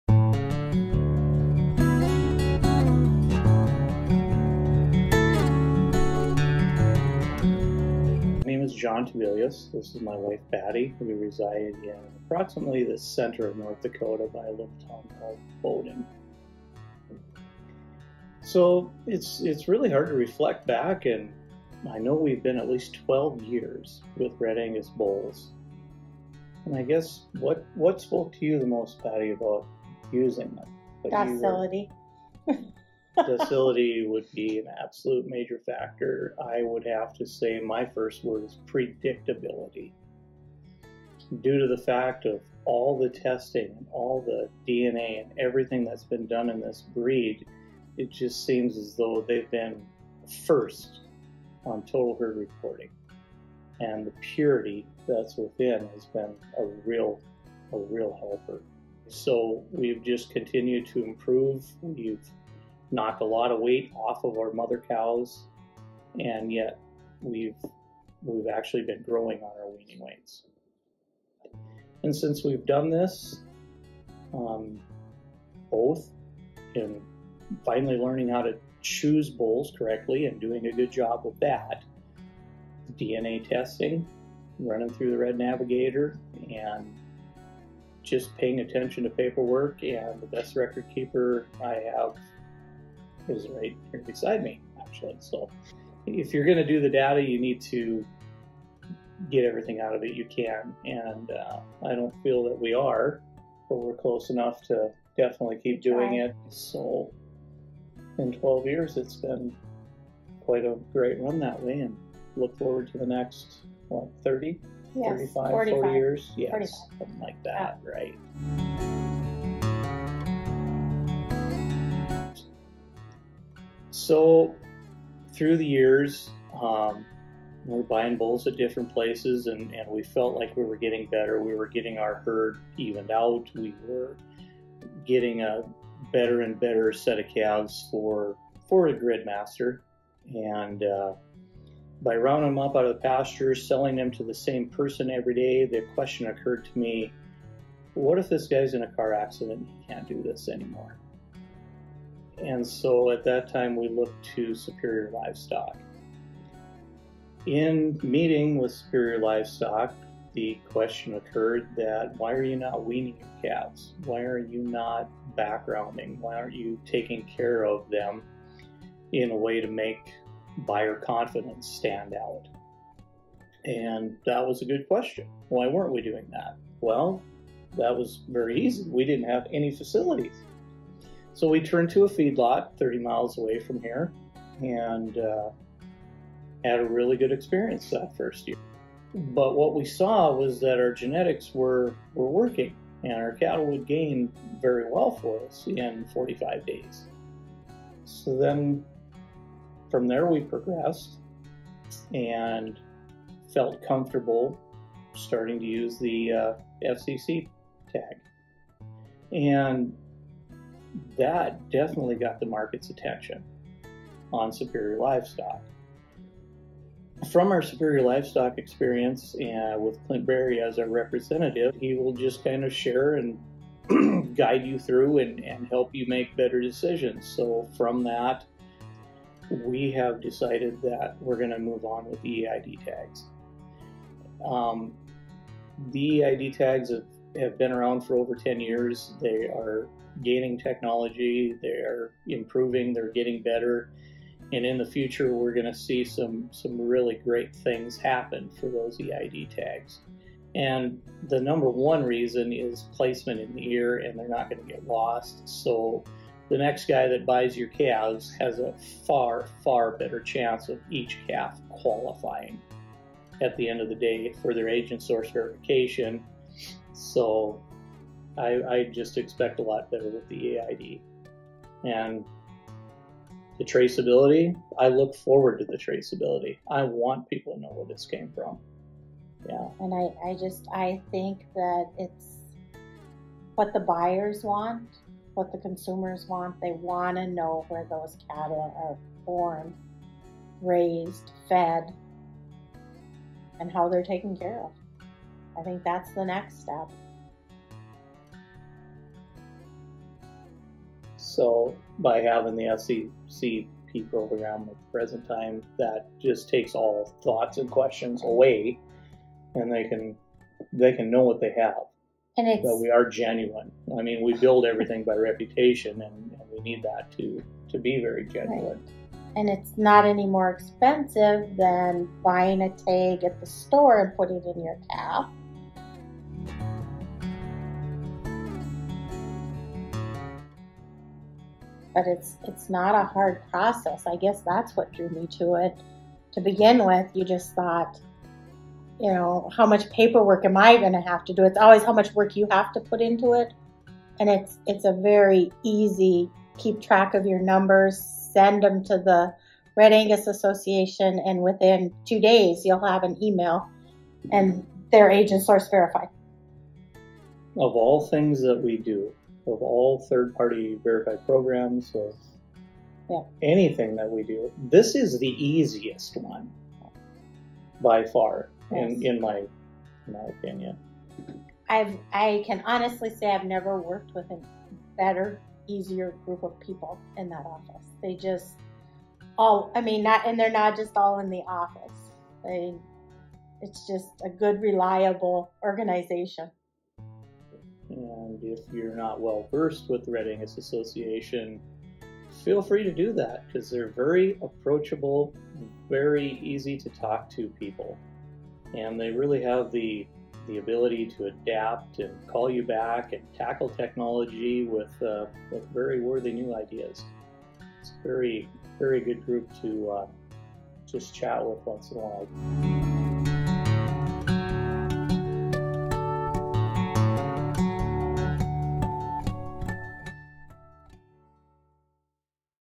Red Angus Tag Testimonial - The Tebelius Ranch
John and Patty Tebelius share the impact RAAA FCCP tags have had in improving record keeping and traceability on their operation.